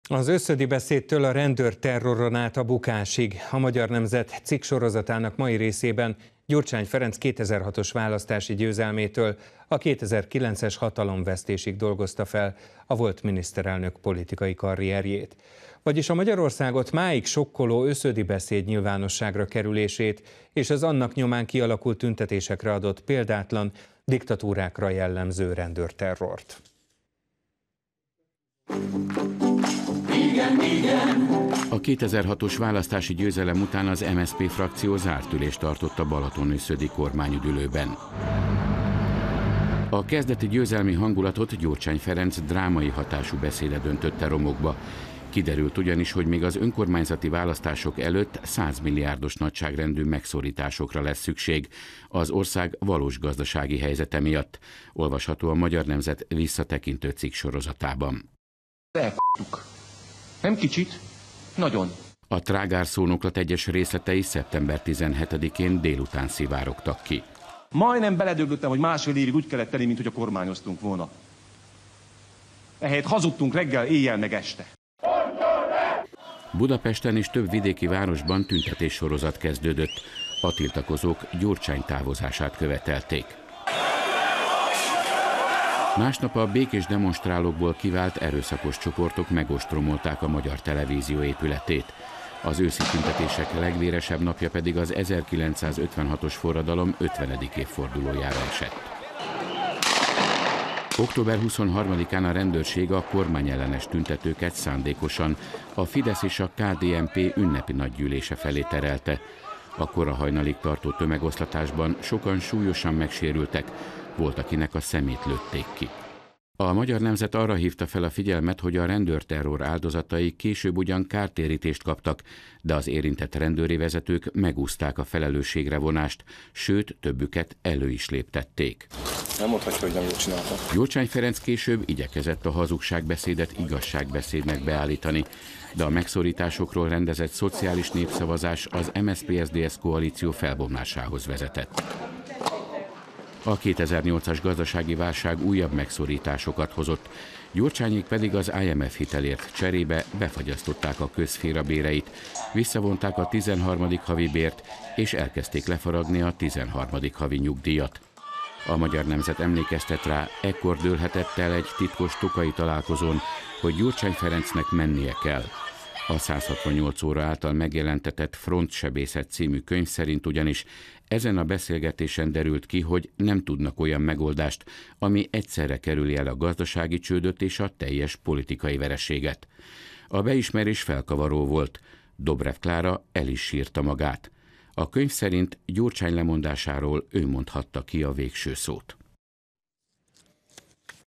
Az őszödi beszédtől a bukásig
Az őszödi beszédtől a rendőrterroron át a bukásig. A Magyar Nemzet cikksorozatának mai részében Gyurcsány Ferenc 2006-os választási győzelmétől a 2009-es hatalomvesztésig dolgozta fel a volt miniszterelnök politikai karrierjét. Vagyis a Magyarországot máig sokkoló őszödi beszéd nyilvánosságra kerülését és az annak nyomán kialakult tüntetésekre adott példátlan, diktatúrákra jellemző rendőrterrort.